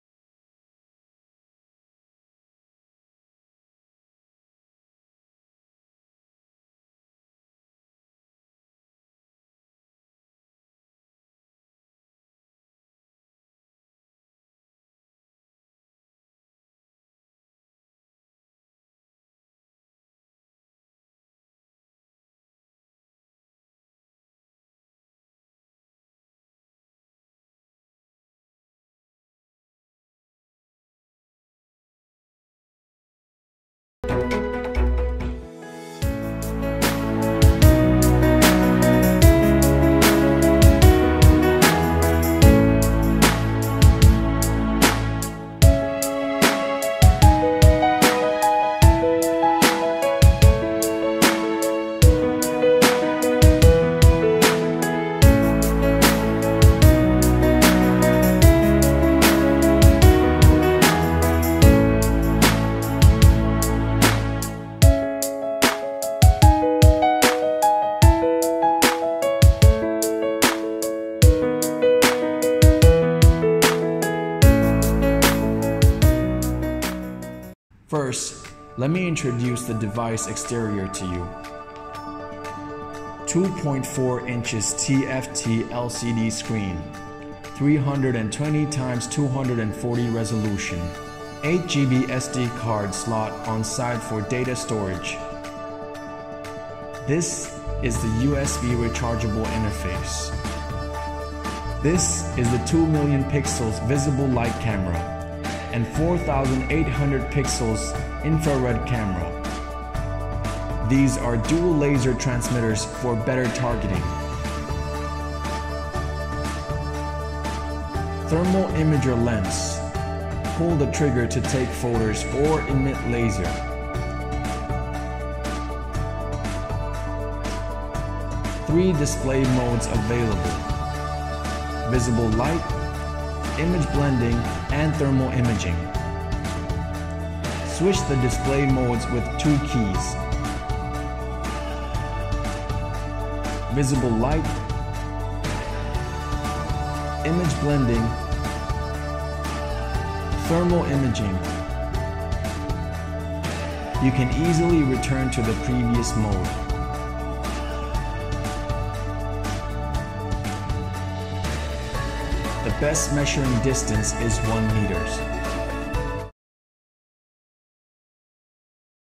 UNI-T UTi80 Industry Thermal Imager -30℃~400℃ (320 x 240 pixels)
Traditional infrared thermometers can only measure single temperature points. UTi80 thermal imager can detect temperature variations in an area through a 2.4-inch LCD display. UTi80’s dual lasers can quickly lock in measurement areas and display target temperature. The standard package also includes an 8GB Micro SD card for image and temperature data storage. UTi80’s image blending feature is able to combine infrared and normal camera images together, allowing in-depth display for areas of interest.

Image modes: Thermal, thermal blending, digital camera
Image frequency: ≤9Hz
Power: Li-ion battery 3.7V 2600mAh